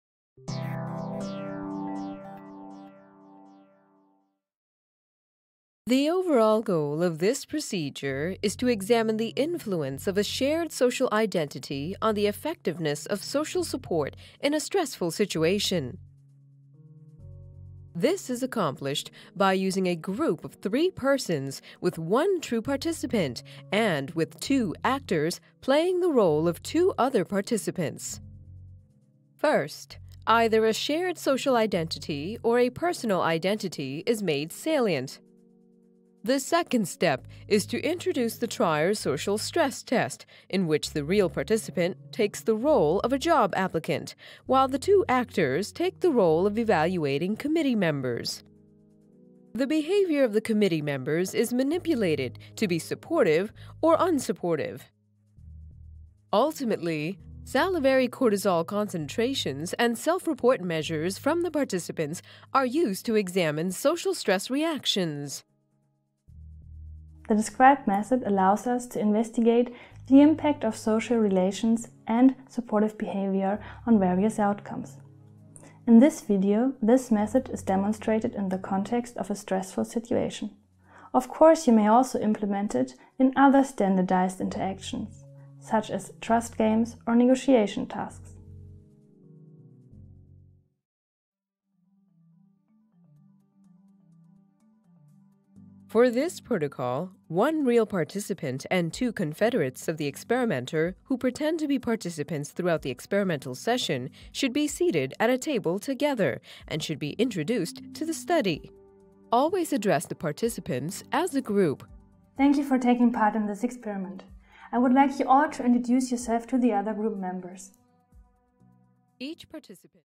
Social Dimension-Stress: Experimental Manipulations-Social Support & Identity l Protocol Preview
The Social Dimension of Stress: Experimental Manipulations of Social Support and Social Identity in the Trier Social Stress Test -  a 2 minute Preview of the Experimental Protocol

Johanna U. Frisch, Jan A. Häusser, Rolf van Dick, Andreas Mojzisch
University of Hildesheim, Institute of Psychology; Goethe-University Frankfurt, Department of Psychology and Sports Science and Center for Leadership and Behavior in Organizations;

Previous research on the social dimension of stress has focused on two important variables: social identity and social support. This protocol introduces an effective experimental manipulation of these two social variables and describes their implementation in a standard stress induction paradigm (Trier Social Stress Test).